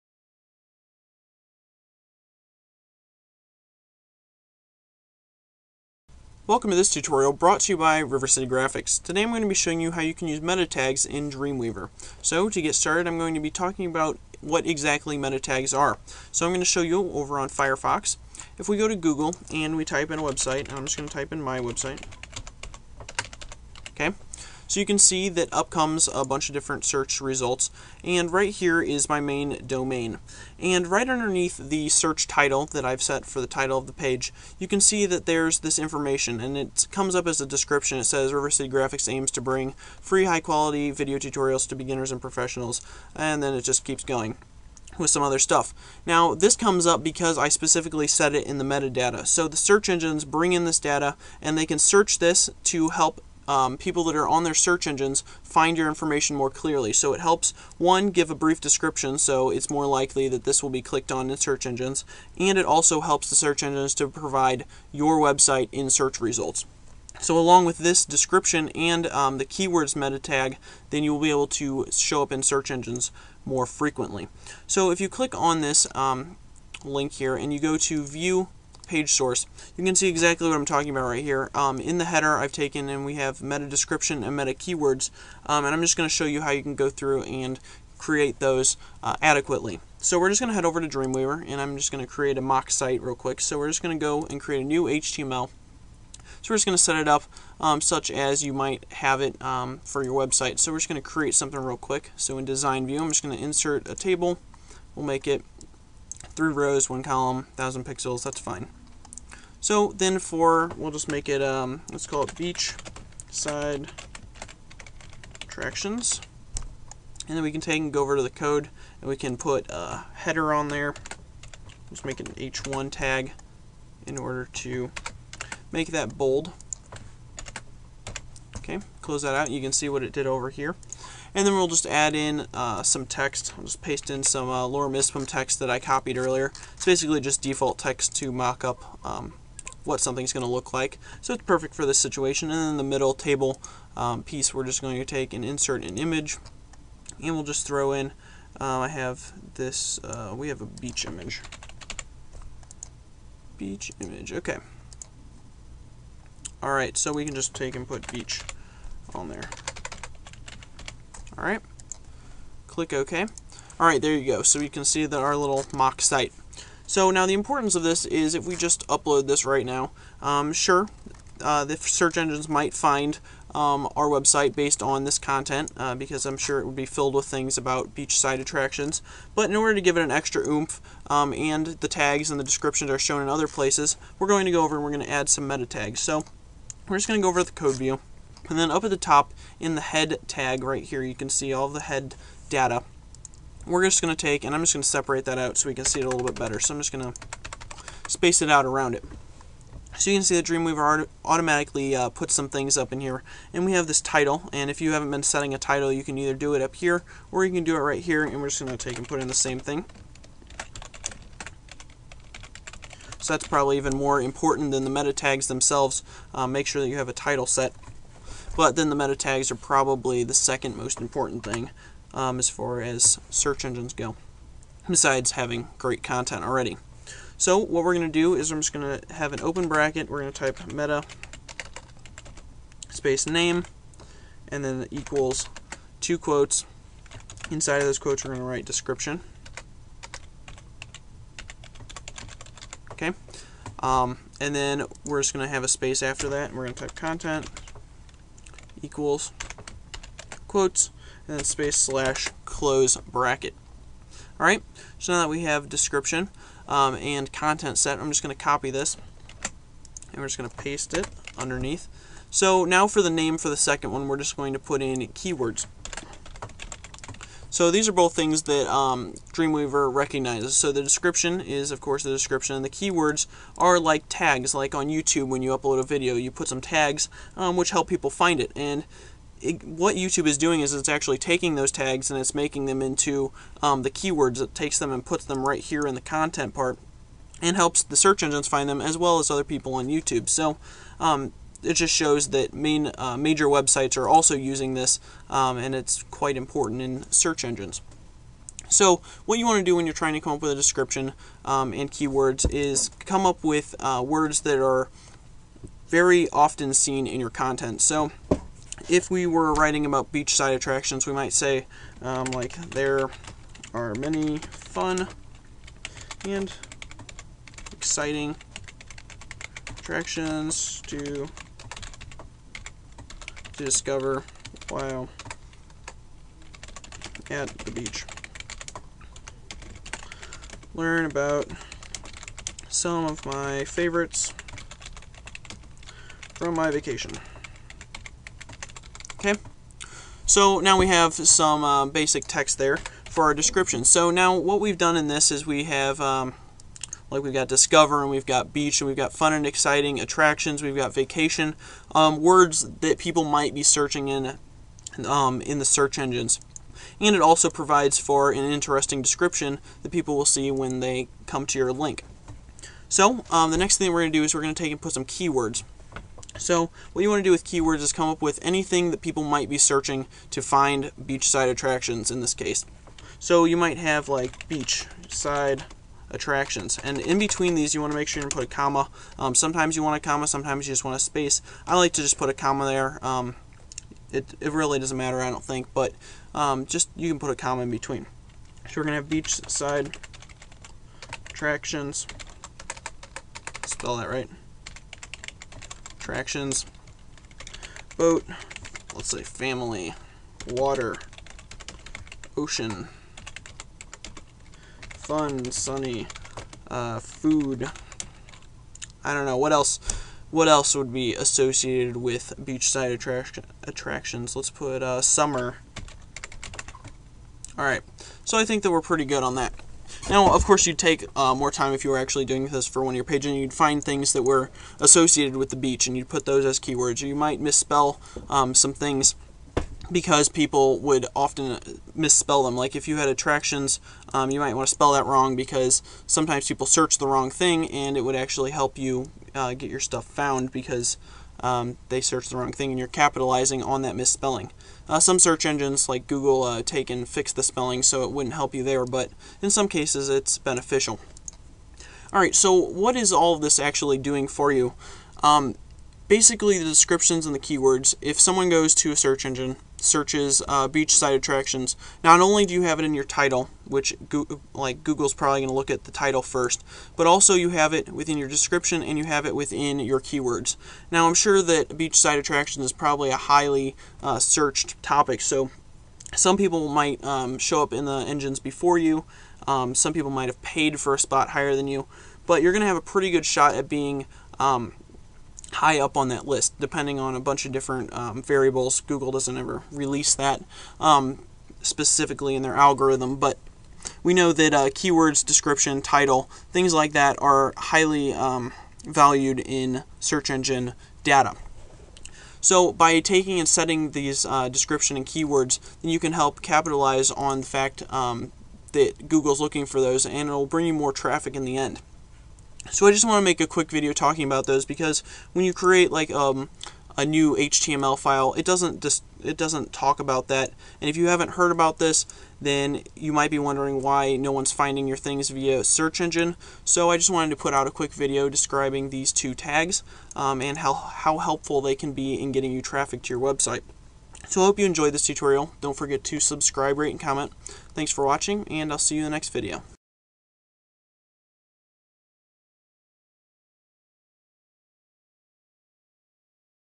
Dreamweaver Tutorial: Meta Tags and Search Engine Optimization -HD-
In this tutorial, you will learn how to add meta tags Dreamweaver CS5 that will allow your web pages to be better found by search engines.